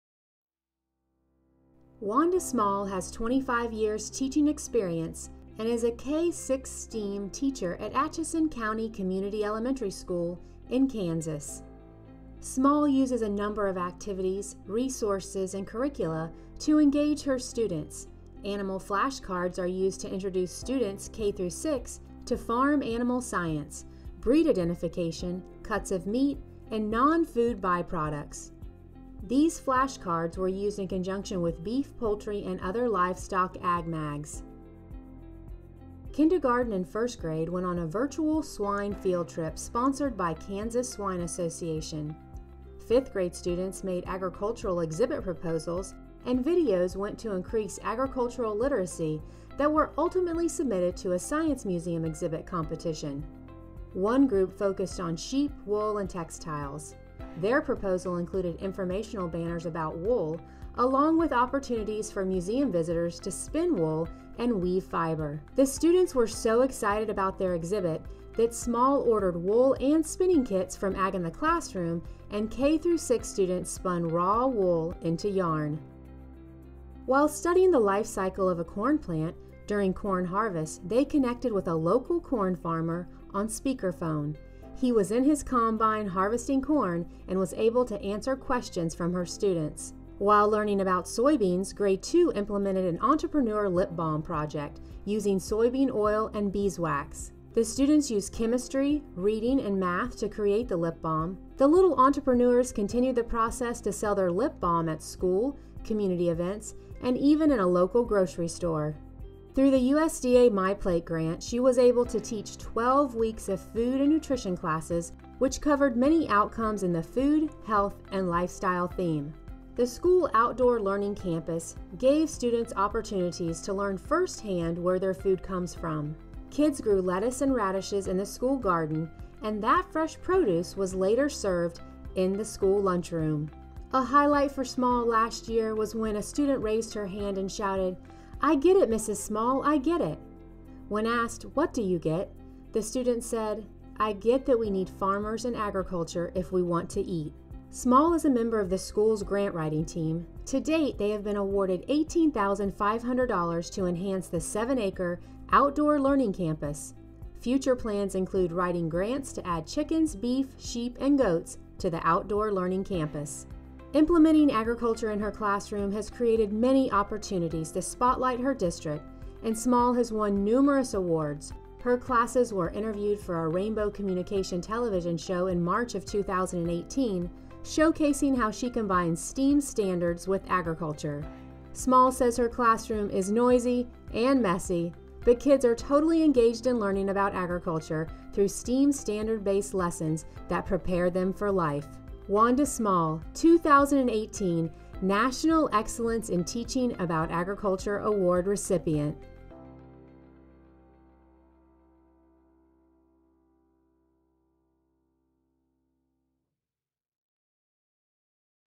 2018 National Excellence in Teaching about Agriculture Award - Kansas
2018 EITAA winner Wanda Small partners with Kansas Foundation for Agriculture in the Classroom, 4-H and Kansas Farm Bureau to educate students about the animal and plant sciences.